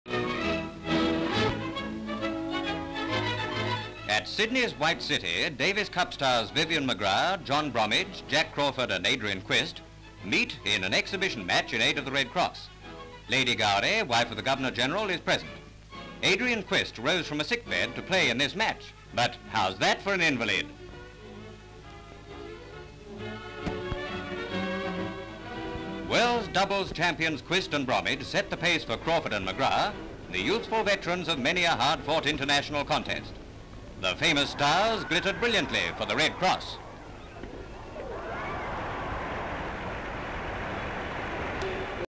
Tennis Match For Red Cross (1940)
Full title reads: "Tennis Match For Red Cross".

White City, Sydney, Australia.

VS. of Jack Crawford, Adrian Quist, John Bromwich and Vivian McGrath playing in exhibition doubles tennis match in aid of Red Cross.

MS. Lady Gowrie wife of Governor General watching the game.
 FILM ID:1057.17